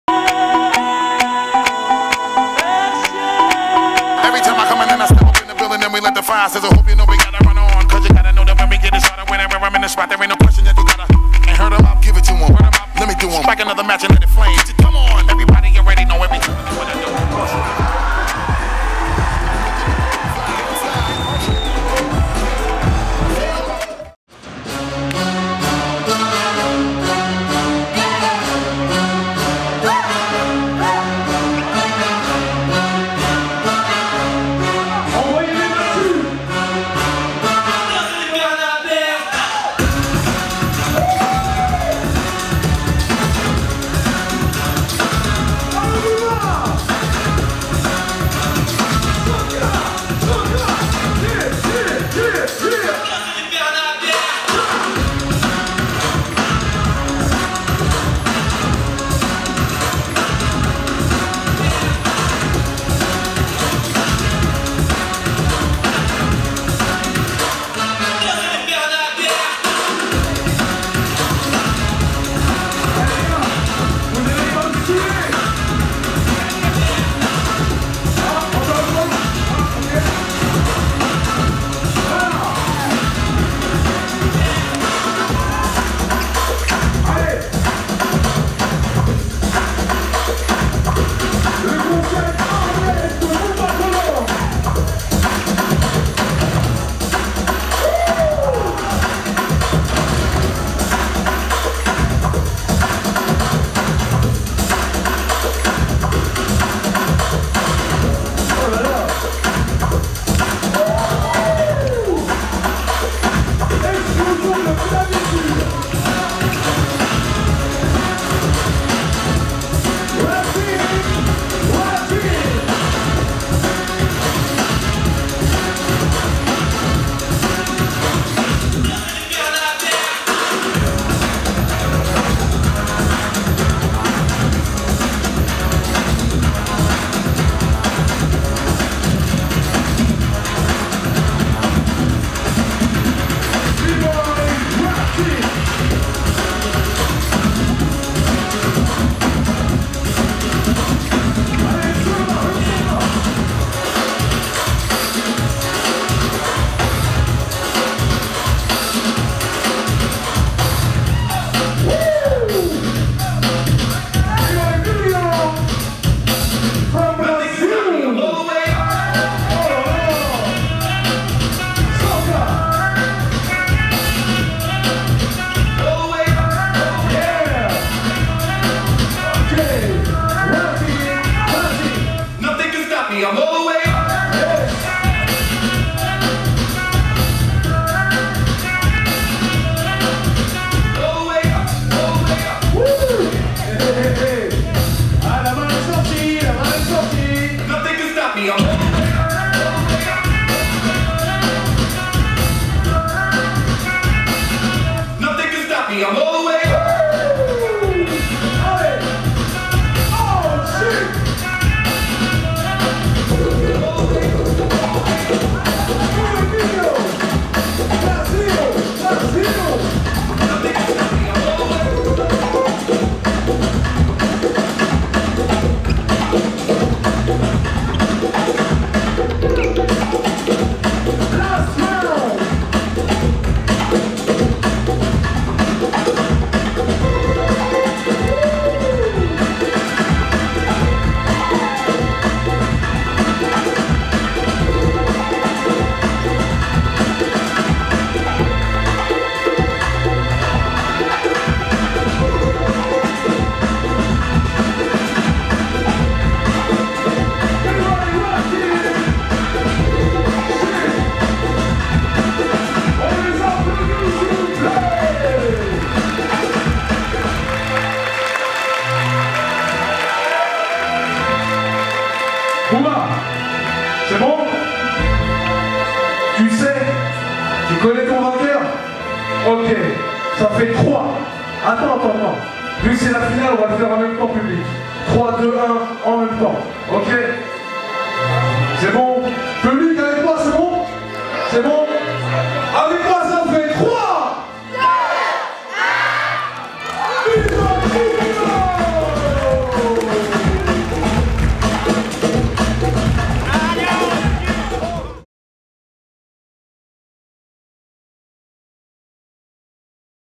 BBOY RATIN (Brazil) vs BBOY DINHO (Brazil) | 1on1 Final Armless | BATTLE RAW CONCEP #7
BBOY RATIN (Brazil) vs BBOY DINHO  (Brazil)
1on1 Final Armless
BATTLE RAW CONCEP #7

Host : Aziz
Dj : Senka